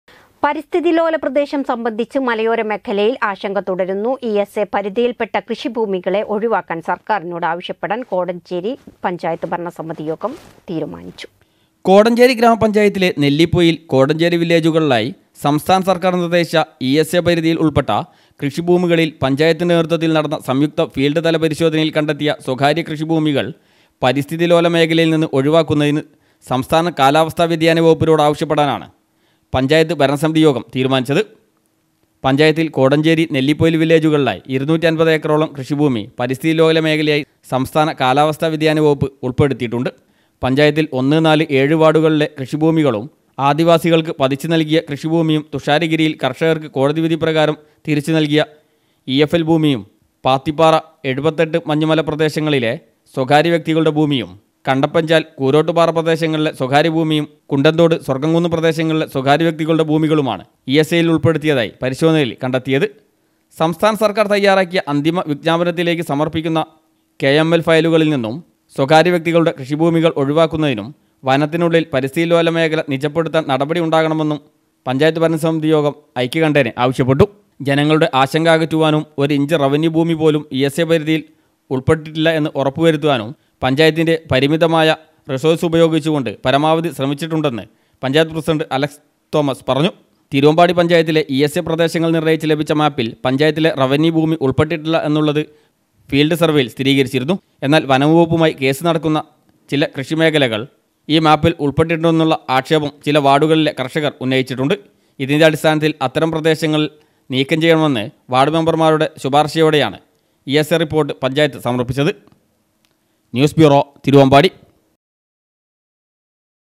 പരിസ്ഥിതി ലോല പ്രദേശം സംബന്ധിച്ച് മലയോര മേഖലയിൽ ആശങ്ക തുടരുന്നു
പരിസ്ഥിതി ലോല പ്രദേശം സംബന്ധിച്ച് മലയോര മേഖലയിൽ ആശങ്ക തുടരുന്നു. ഇ എസ് എ പരിധിയിൽ പെട്ട കൃഷിഭൂമികളെ ഒഴിവാക്കാൻ സർക്കാരിനോട് ആവശ്യപ്പെടാൻ കോടഞ്ചേരി പഞ്ചായത്ത് ഭരണസമിതി യോഗം തീരുമാനിച്ചു